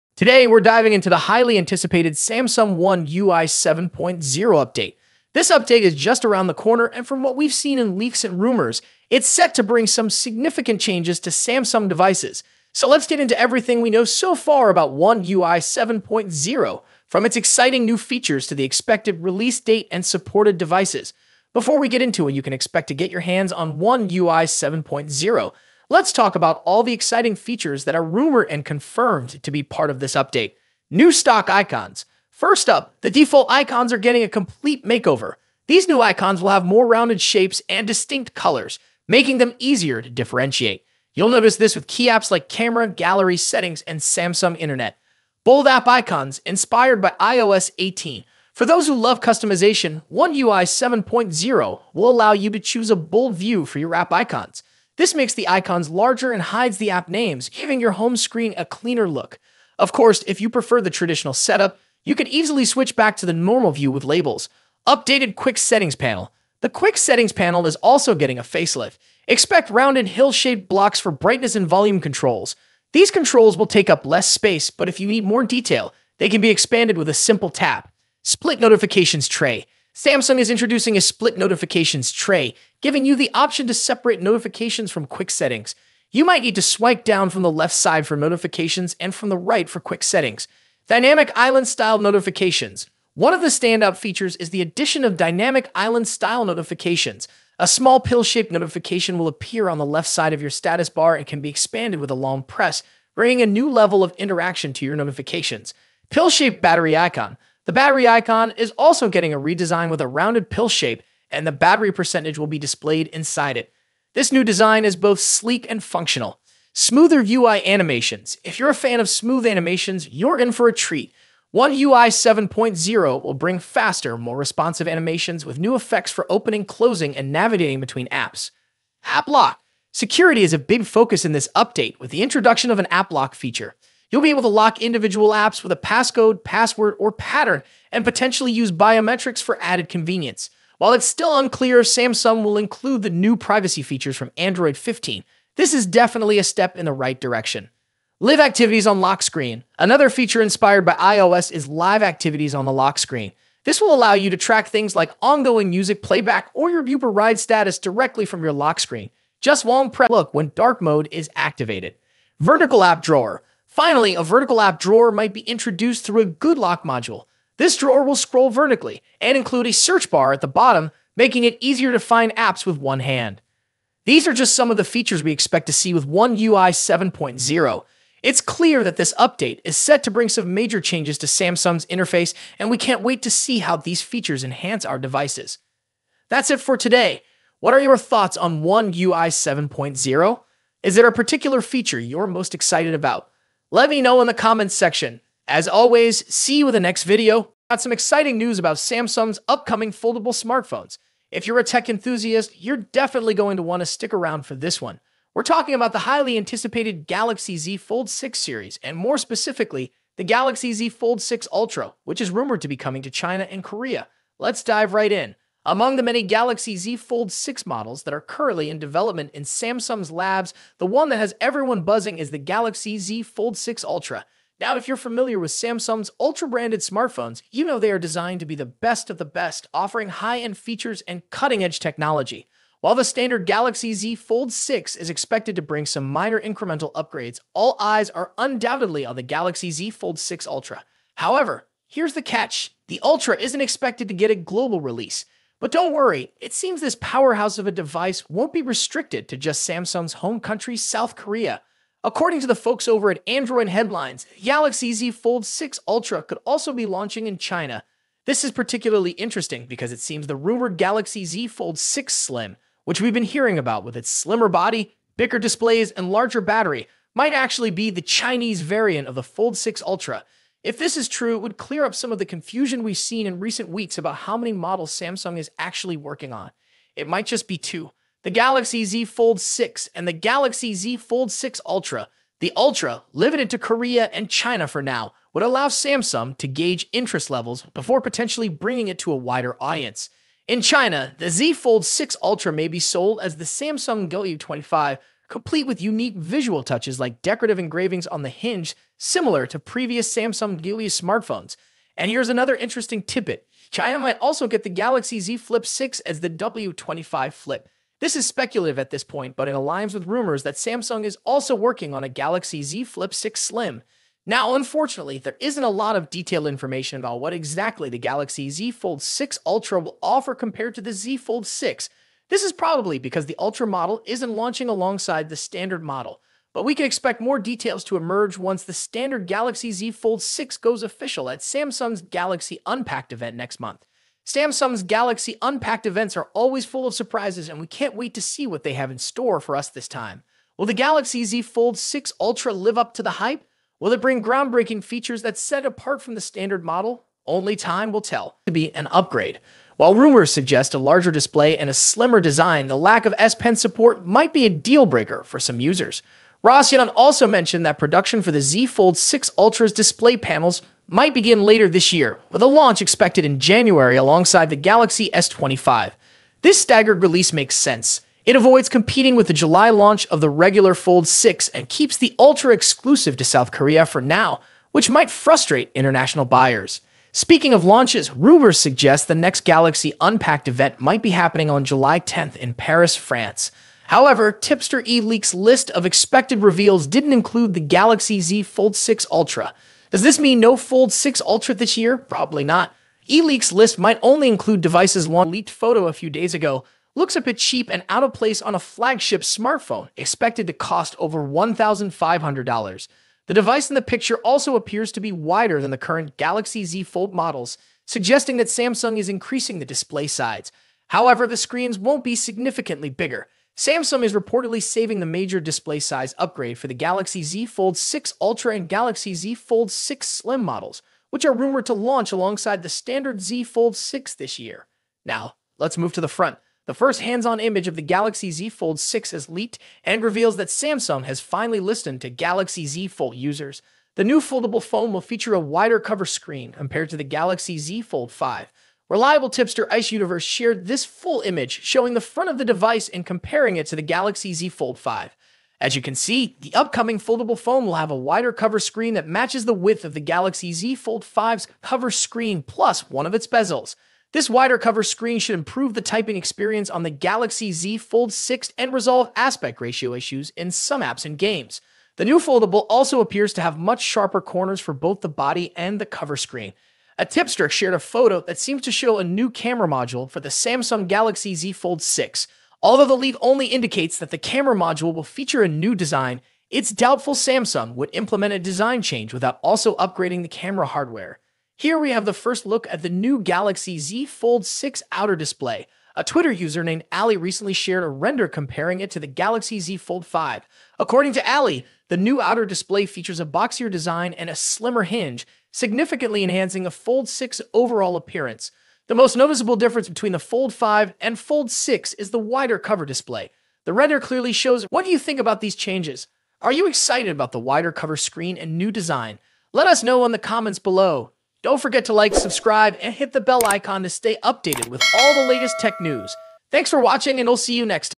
Samsung One UI 7.0 Android 15 - TOP 15 NEW FEATURES!!
Here we have the list of the Top 15 New Features of Samsung One UI 7.0 Android 15, Release Date & Supported Devices details and more...

What do you think about Samsung One UI 7.0 Android 15?

PEACE!!